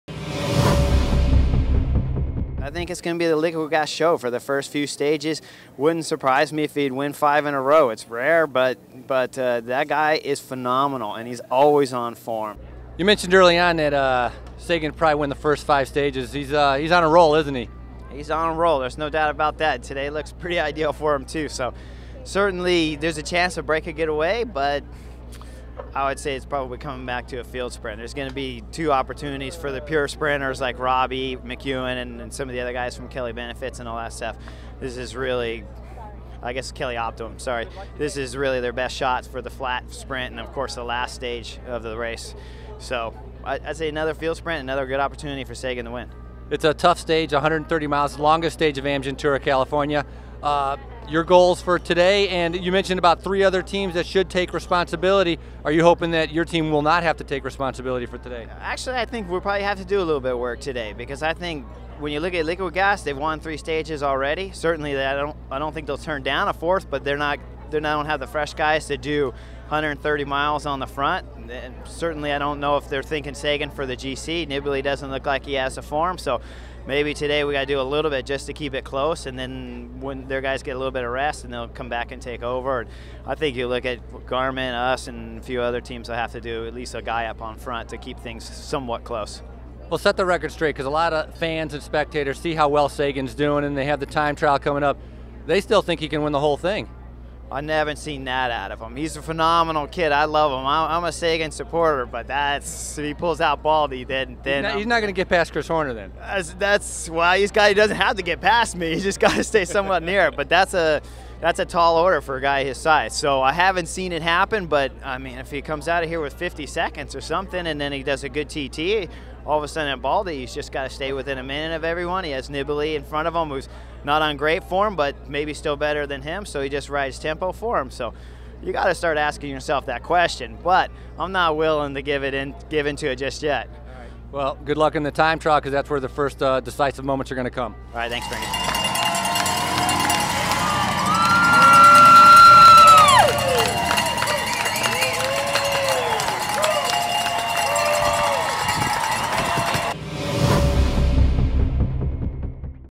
Chris Horner on Stage 4 Amgen Tour of California - Bicycling Magazine
Chris Horner discusses Amgen Stage 4 with Bicycling magazine.